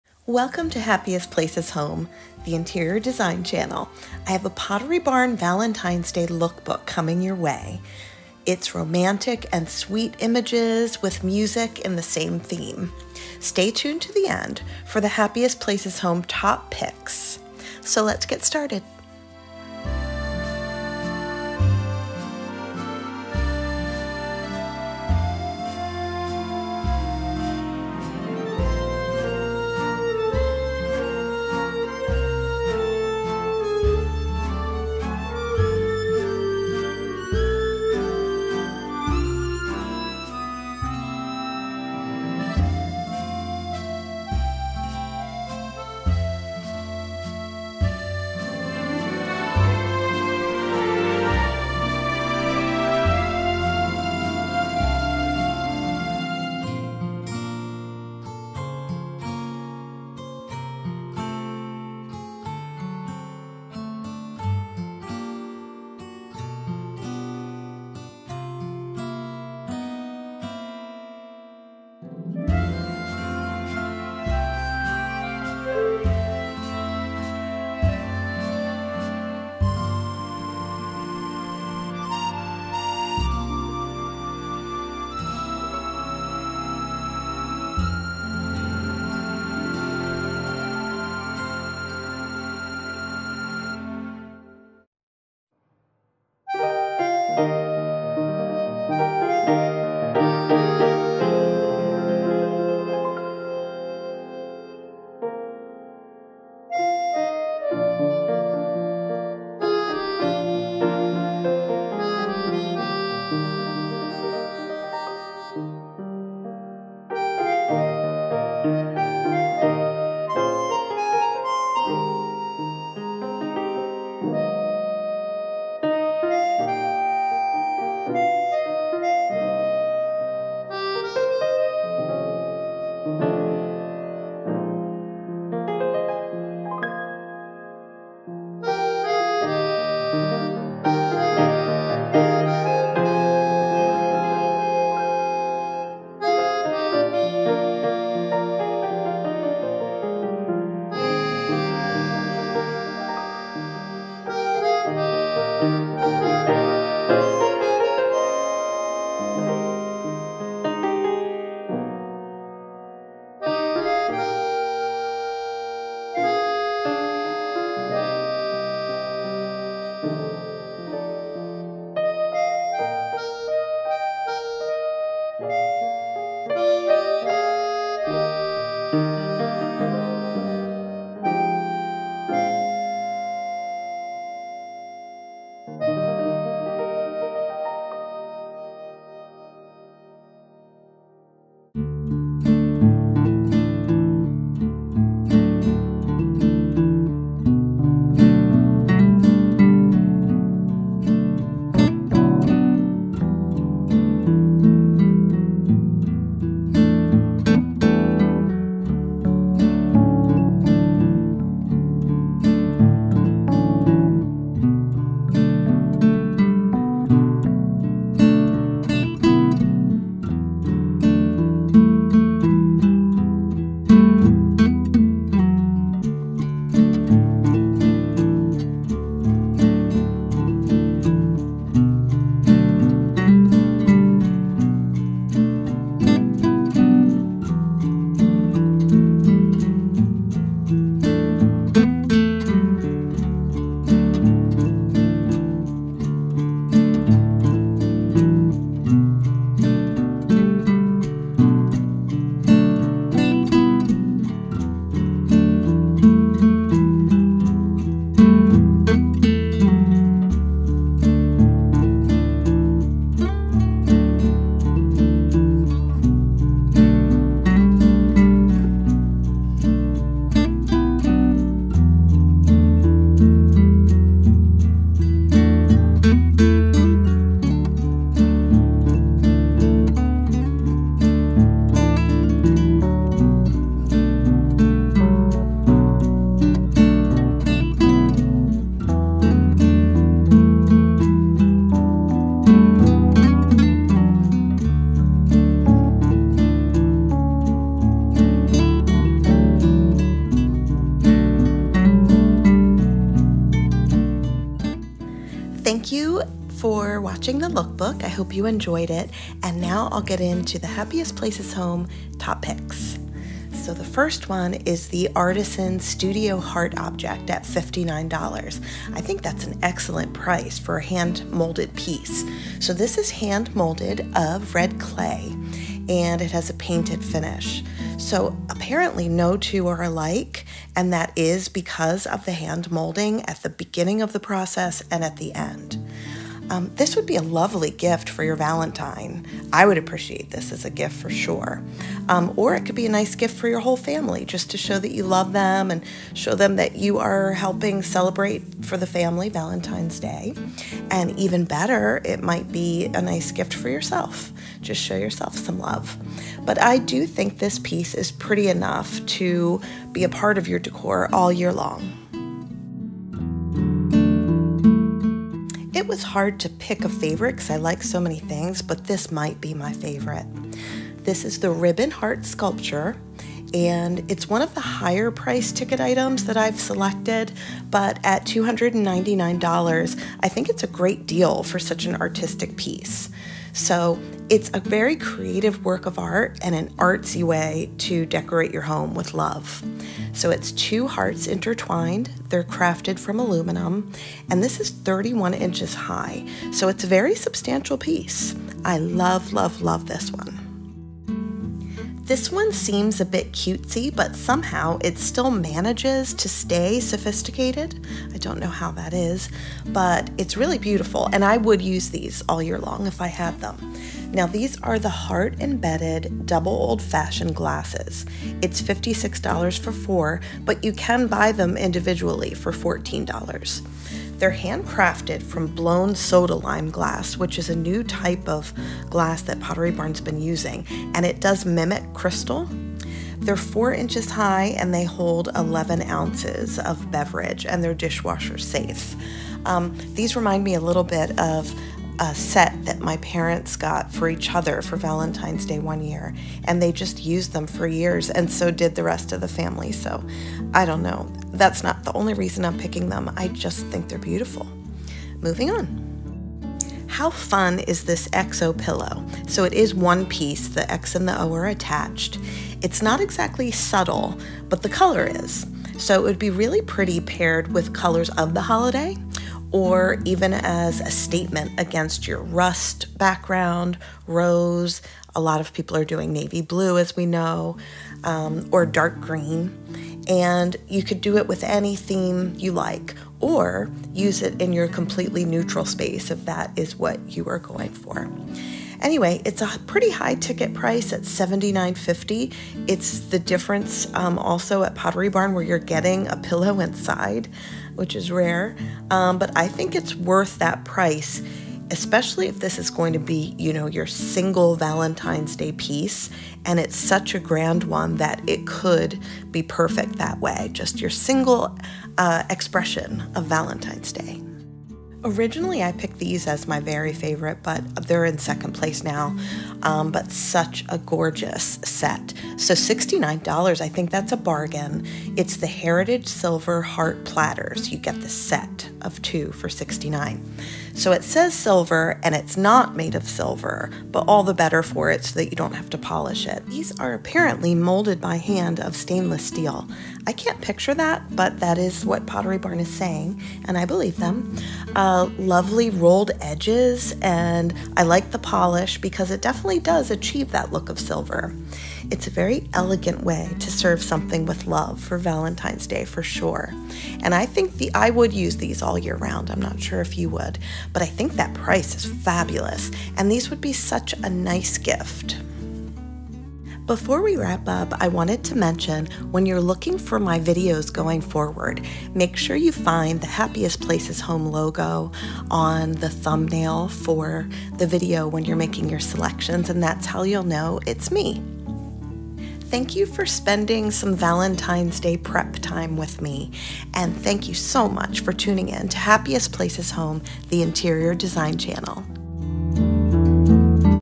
Pottery Barn Valentine’s Day LOOKBOOK!
2024 Valentine’s Day Decor from Pottery Barn!  Valentine’s Day Decorating ideas!  Happy Valentine’s Day!  I hope you enjoy this Pottery Barn LOOKBOOK!